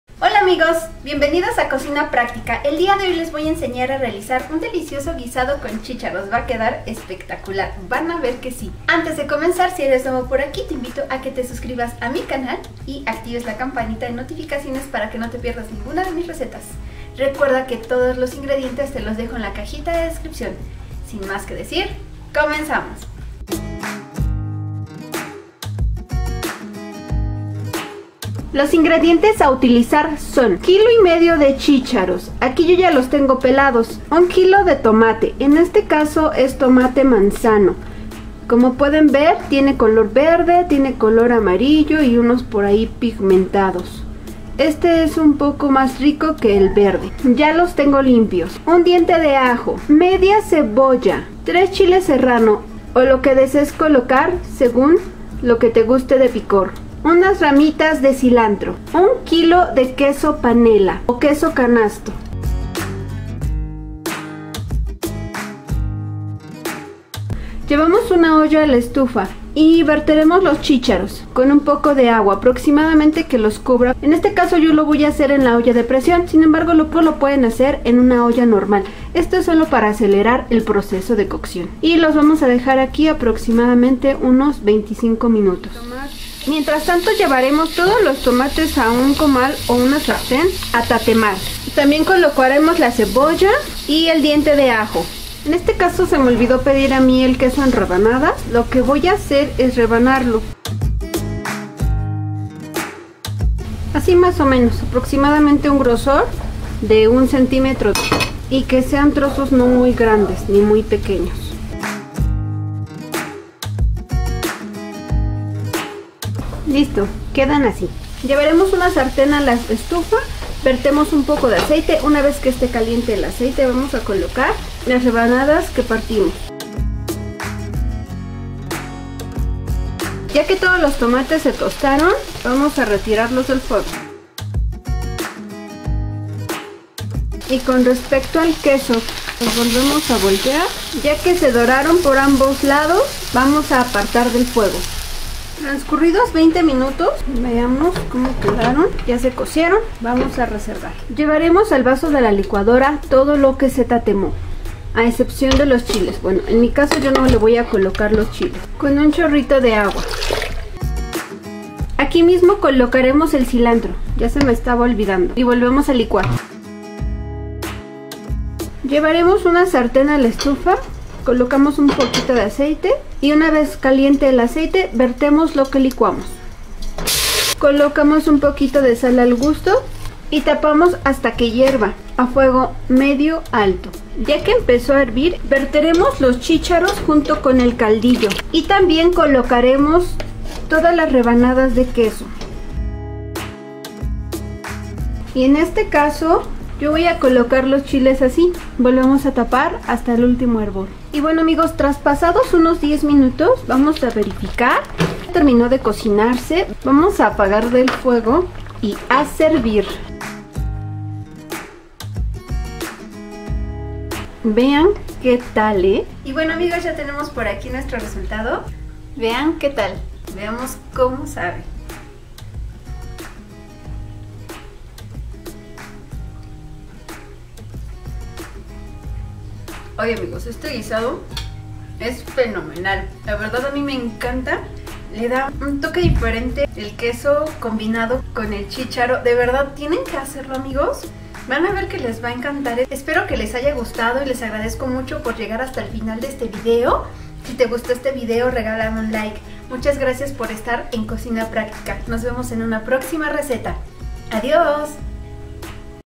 GUISADO DE CHÍCHAROS | cocina práctica con Nancy Miroslava
prepara este delicioso guisado con los siguientes ingredientes:

-1 1/2 kg de chicharos ya pelados son aproximadamente 1kg
-1 kg de tomate manzano
1 diente de ajo
-1/2 de una cebolla
-chiles serrano a elección
-2 ramitas de cilantro
-1 kg de queso panela o queso canasto

¡manos a la obra y a disfrutar!